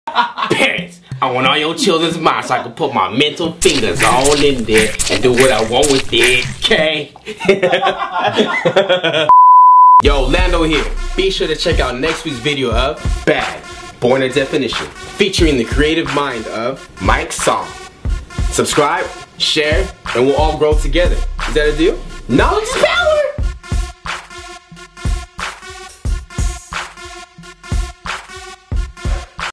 BADnuz || MIKE SONG - B.A.D. || BORN A DEFINITION COMMERCIAL EP.1
Next week will be the first release of the all new B.A.D. BORN A DEFINITION series featuring every bodies creative favorite MIKE SONG. Be sure to check in and spread the awareness cause this year is all about growth for the GENERATION LOST. Find yourself in someone if you cannot do it alone.

*NOTE* THIS SERIES DOES CONTAIN ANY DANCE. THIS IS PURELY MOTIVATIONAL AND INTELLECTUAL TO SPARK INNOVATION AND CREATIVE THOUGHT FOR THOSE WHO ARE LESS FORTUNATE AT NOT HAVING ACCESS TO RESOURCES THAT WE ALL BENEFIT FROM. PLEASE REFRAIN FROM ANY NEGATIVE COMMENTS OR DISLIKES. SPREAD THE KNOWLEDGE, SHARE GROWTH, AND MOTIVATE YOUR ENVIRONMENT.

Directed by Lando Wilkins

Edited by Manh Media